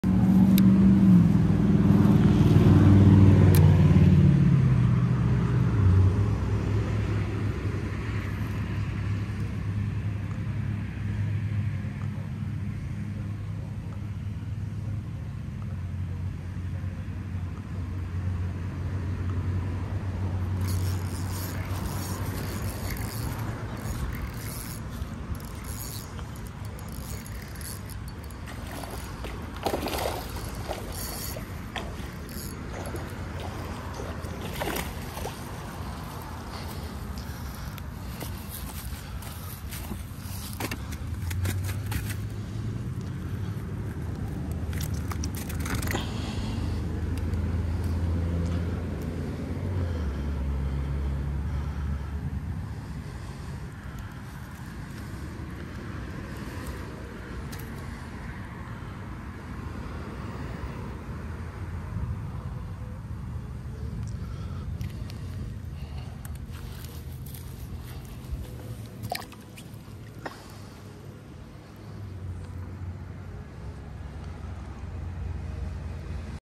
Peacock bass fishing on crappie rod
South Florida peacock bass fishing on Croppie rod with a pound test